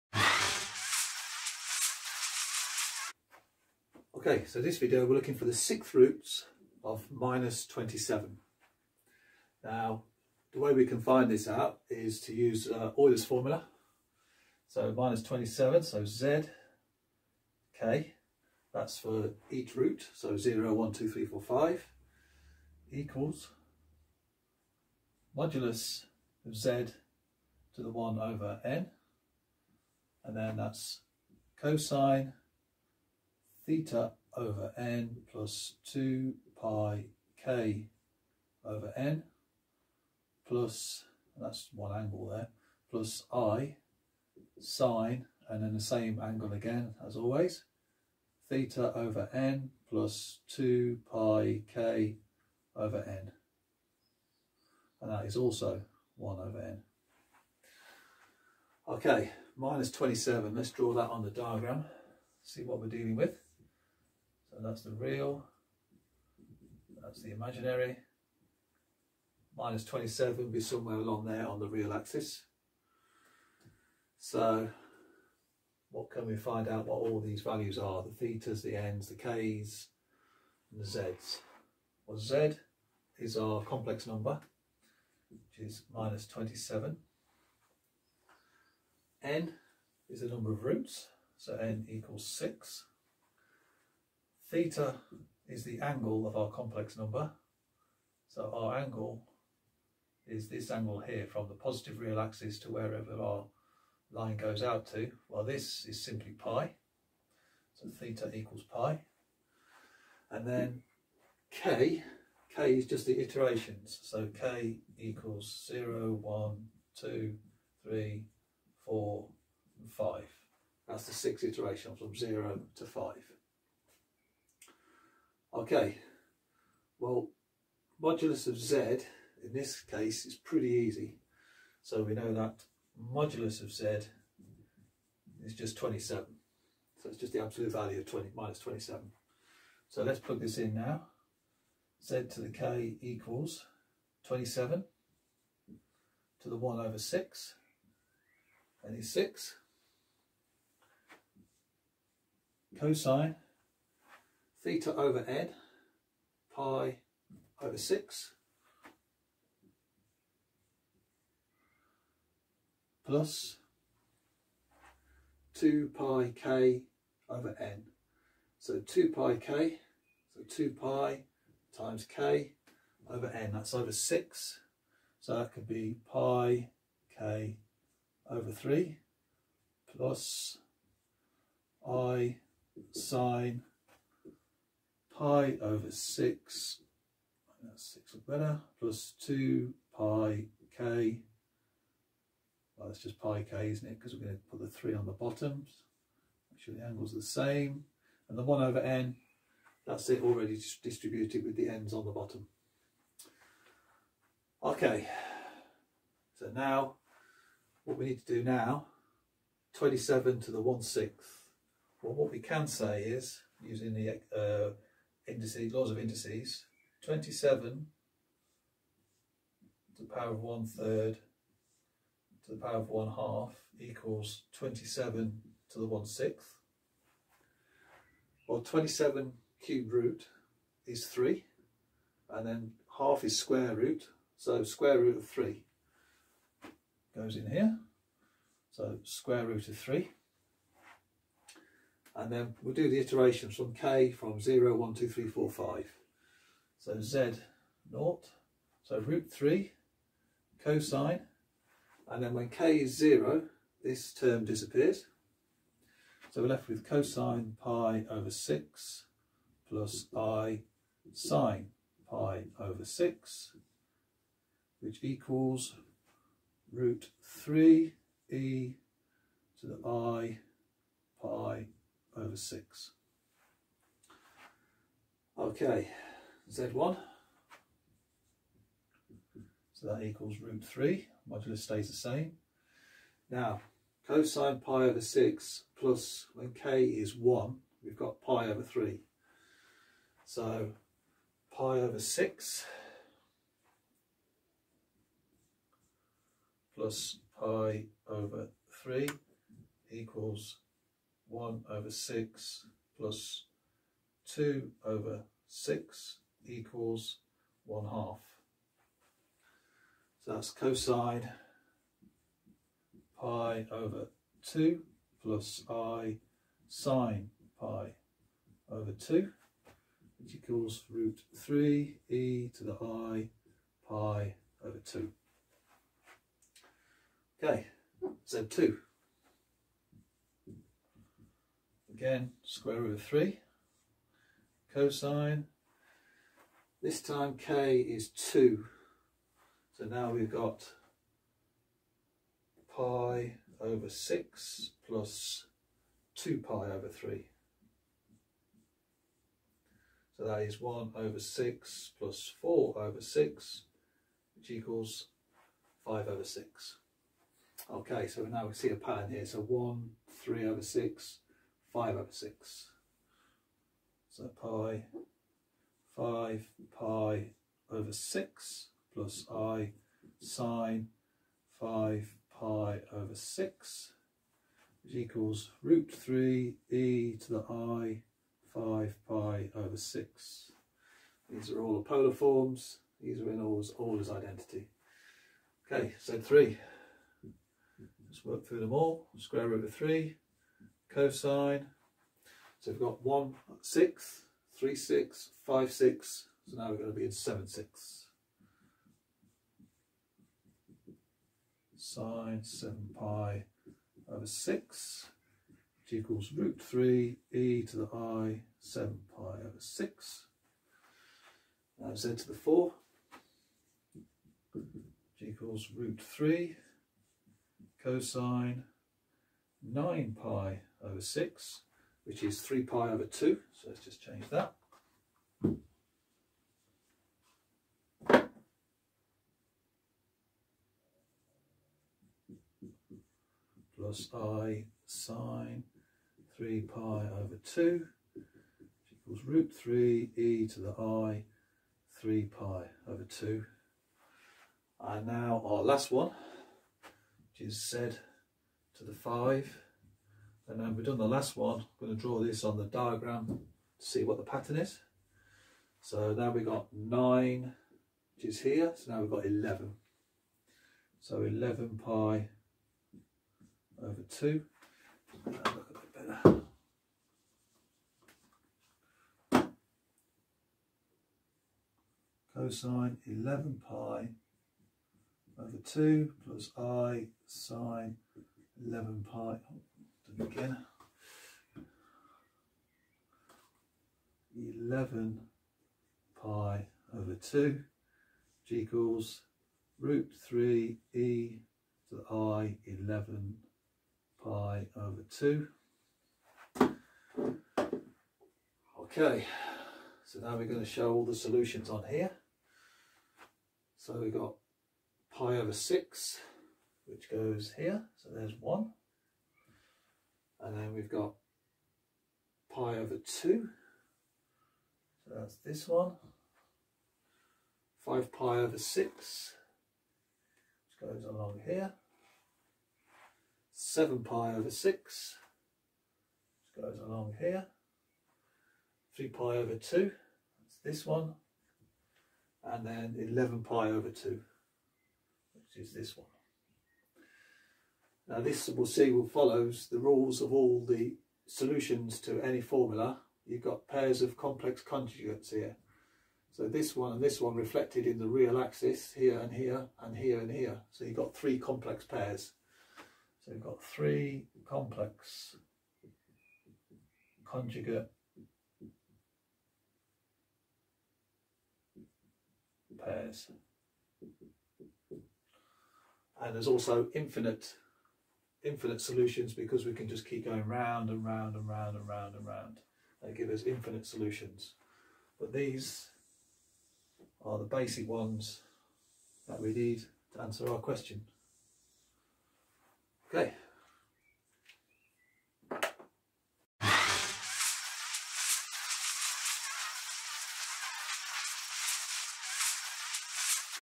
How to find the 6th roots of -27 Complex Analysis
In this tutorial we look for the 6th roots of -27.
First find the radius or magnitude of |-27| = 27 .
Then find the argument which is Pi in this case.
Then using Eulers Formula and De Moivres Formulas we sub in the value n=6 as we need the 6th roots and k=0,1,2,3,4,5 .
The 6th root of 27 is the square root of the cube root . which is square root of 3 .
The answers are shown in Cartesian format and Exponential format .